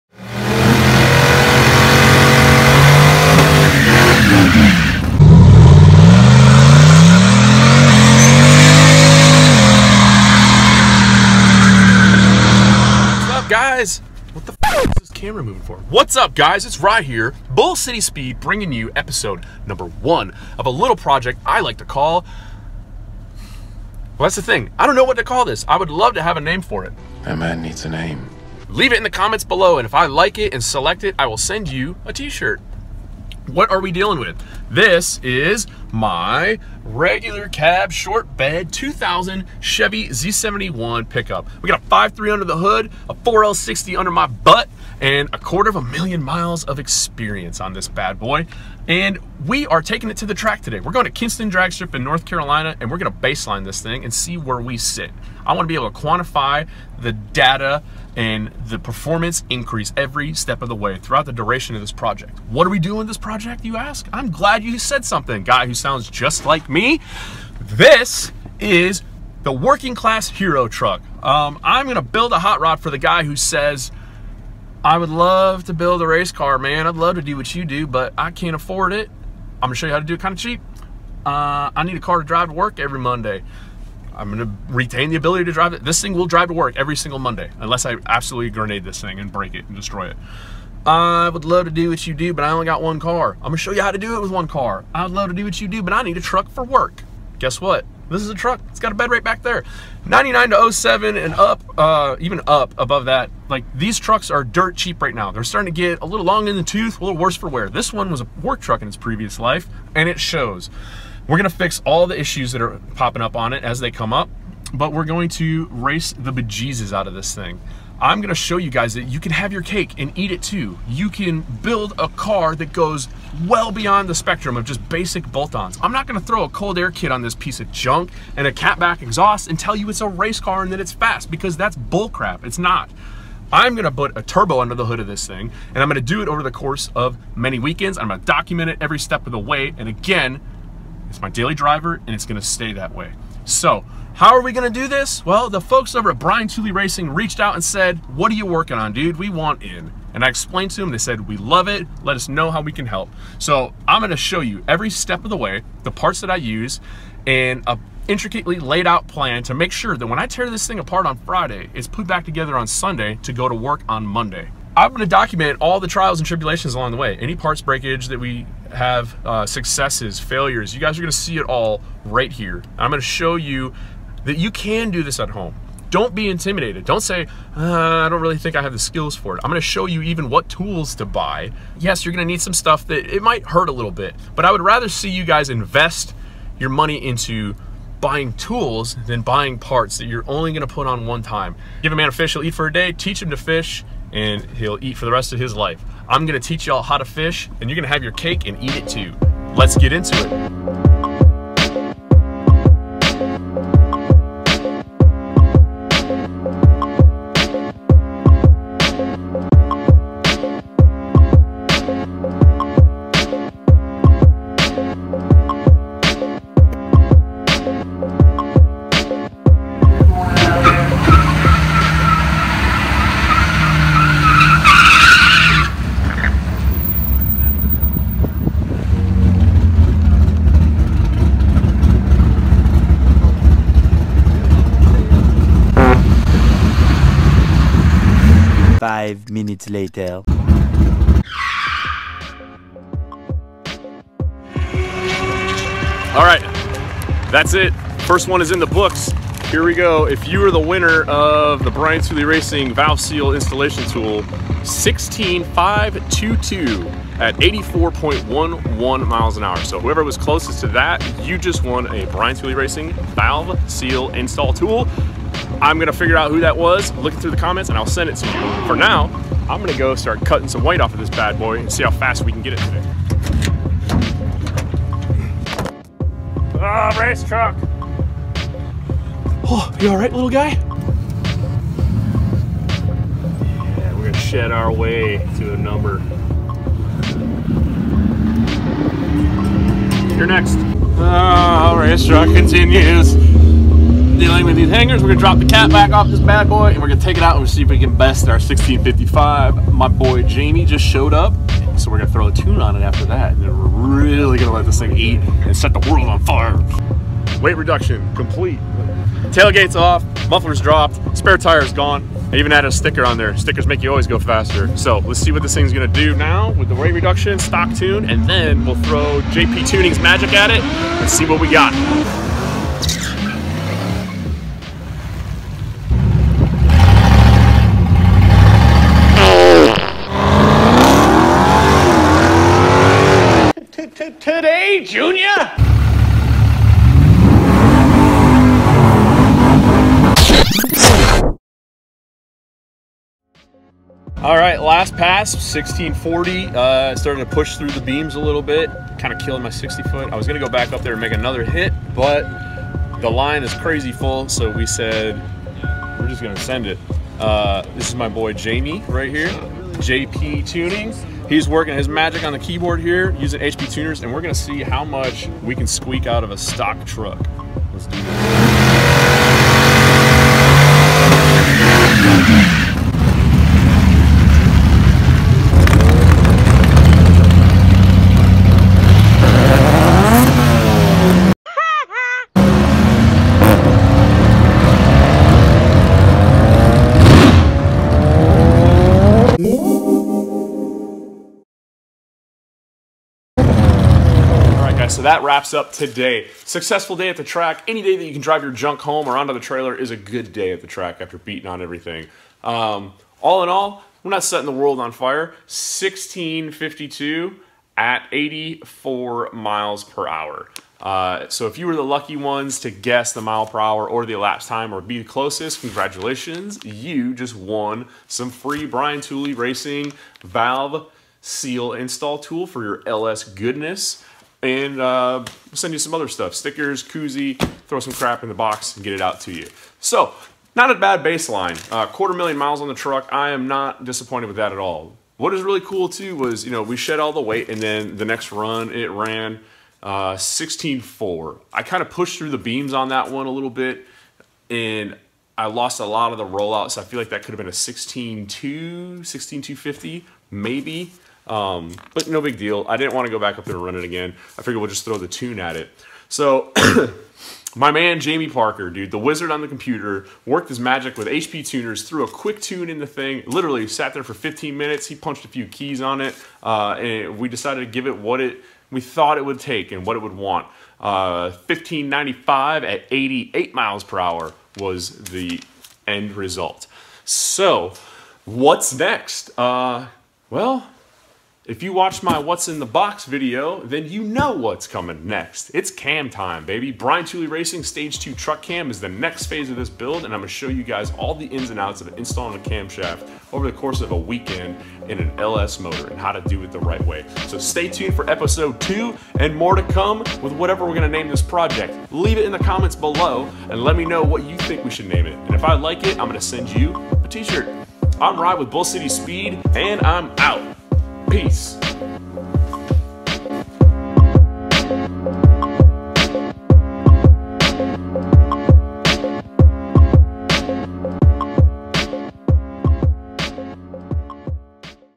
Daily Driven Ep1: Keep on Truckin'
Can Ry make his BeaterDaily RegularCab a reliable street rod? It's got 250,000 miles on the clock, a 4l60, and nothing but tempered spirit under the hood! Let's watch the first episode in the DailyDriven series: KeepOnTruckin' by BTR & Bull City Speed!